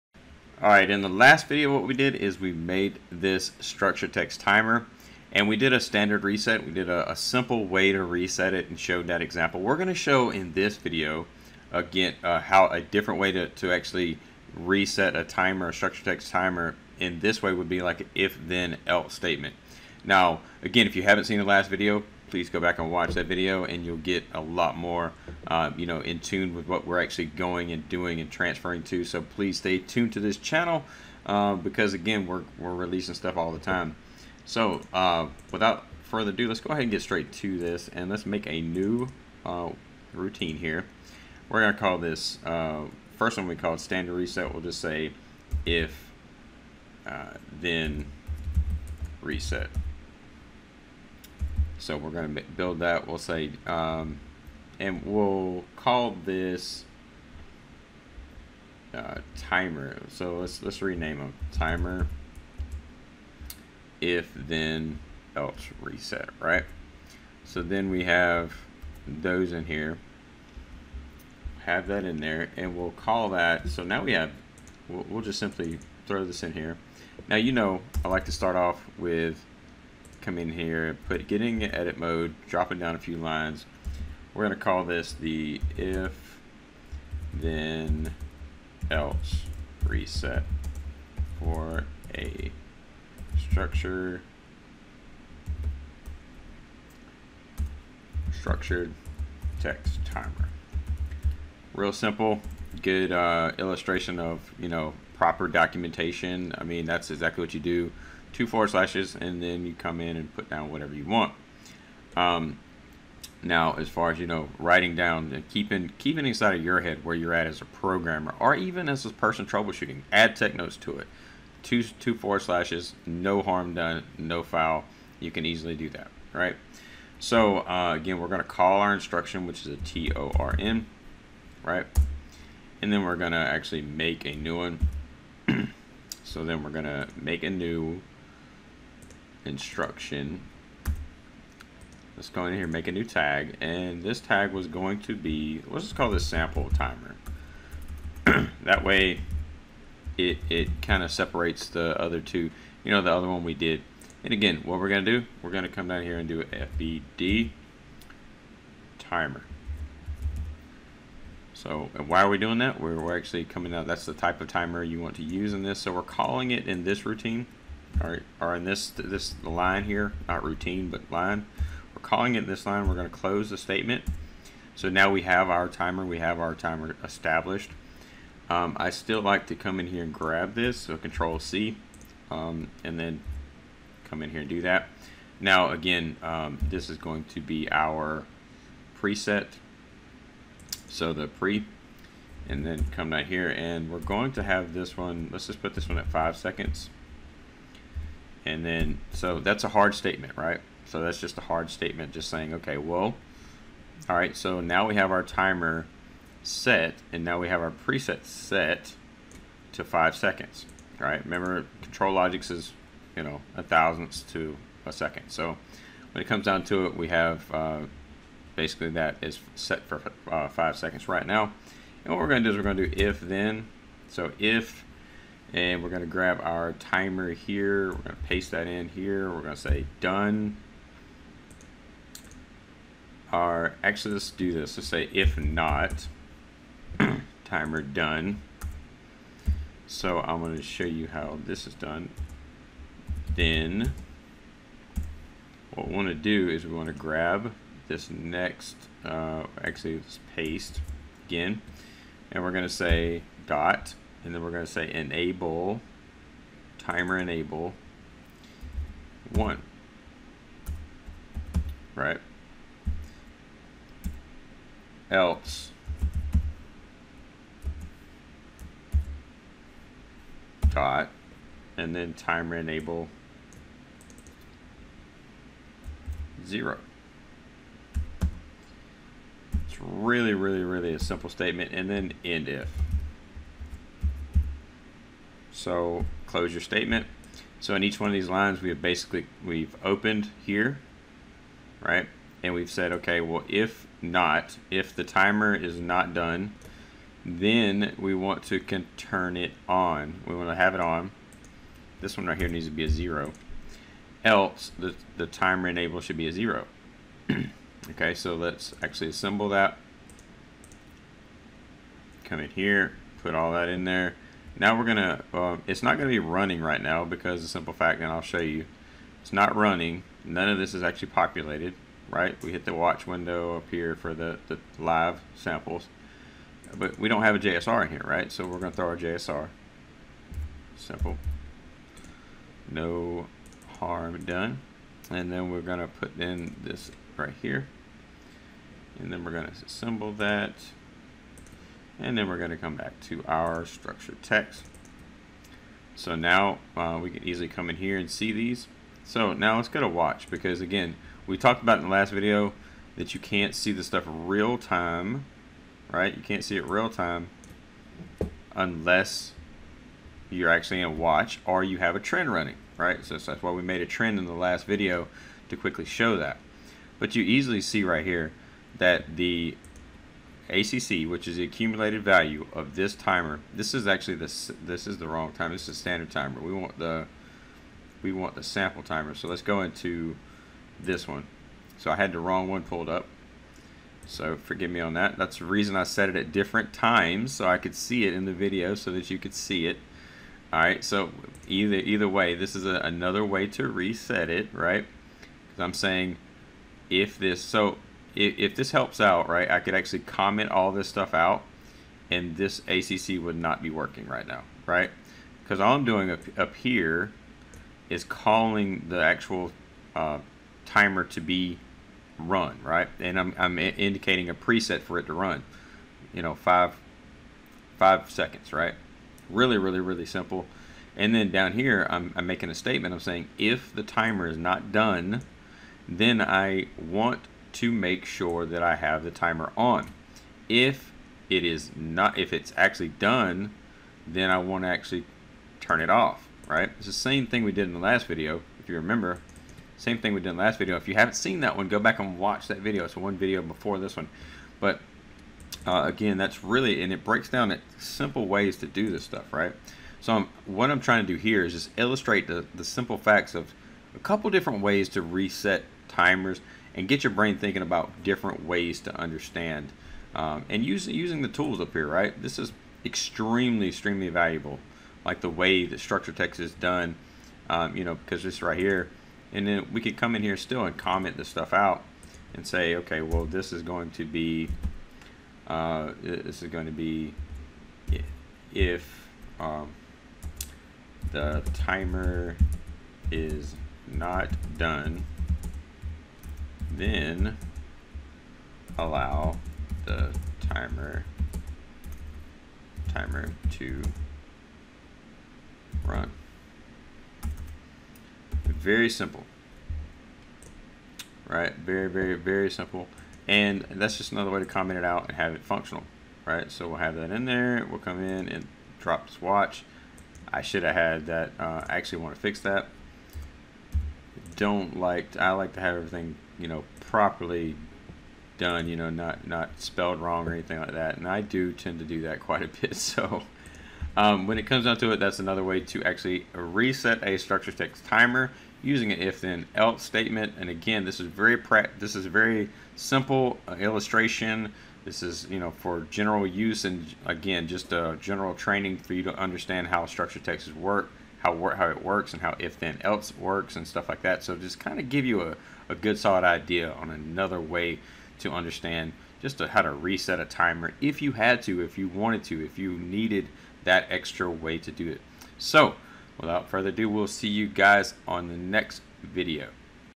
Studio 5000 Version 32 Structured Text (Timer If Then Statement Reset) - TONR Instruction
Programming Structure Text Timer Resets in a different way in Studio 5000 Version 32. This time using an If Then Else statement in the structure text programming language for Rockwell Automation.

A slightly different way to reset a TONR timer in structure text to give you a different way to think about programming.

Automation is our future so with that being said, staying focused on bettering ourselves every day is important.

This is video 2 of a 3 video series for structure text timers & the uses.